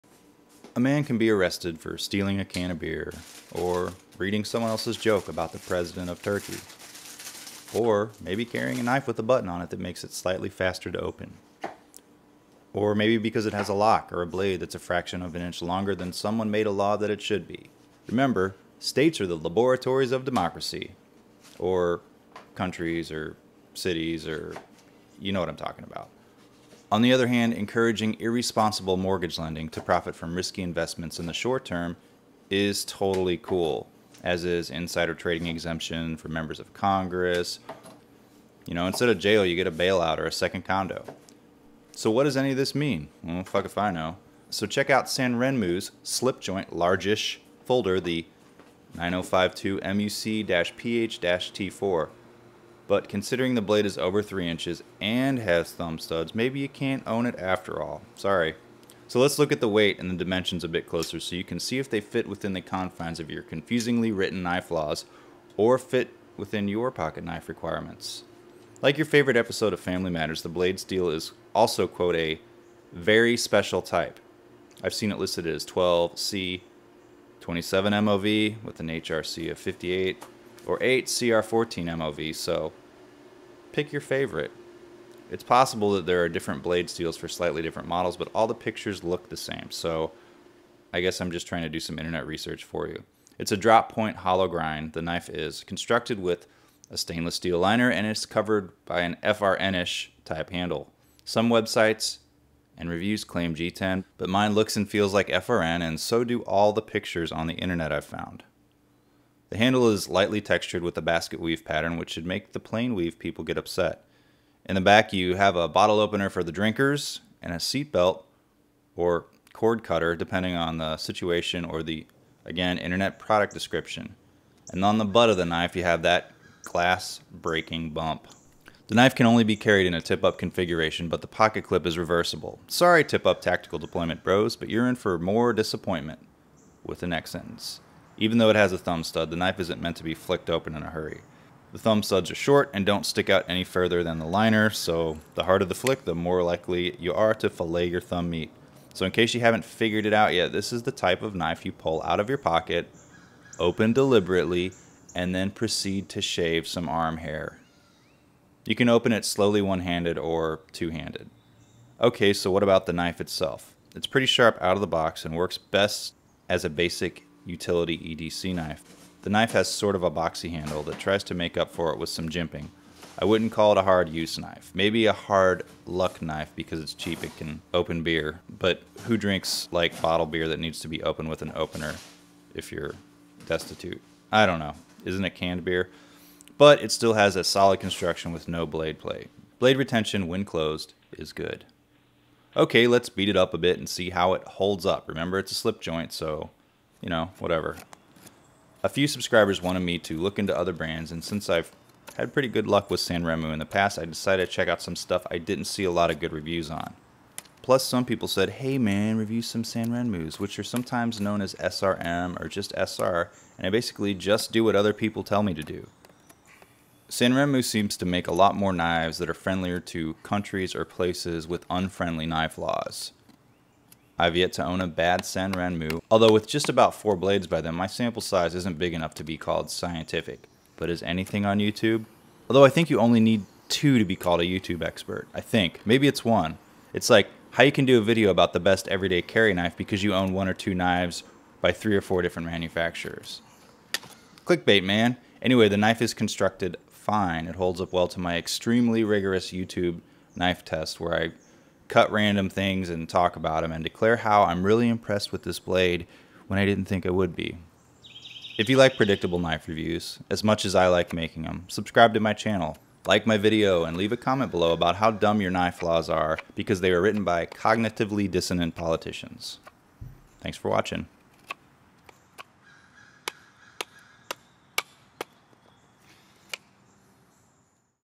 Sanrenmu 9052 Slip Joint Pocket Knife Review (3.3 in blade) Plus wood cutting.
This knife review is for the budget friendly (under $20) Sanrenmu 9052MUC-TH-P4 folding pocket knife.  The knife has a 3.3 inch blade (8.3cm) and it's a slip joint style, which makes the blade legal in more parts of the world.  It's a great cutter, and in this video you'll see me slice tomatoes, cut wood, and slices boxes without having to sharpen the blade.  Also it has a bottle opener and a cord cutter to add to it's overall utility.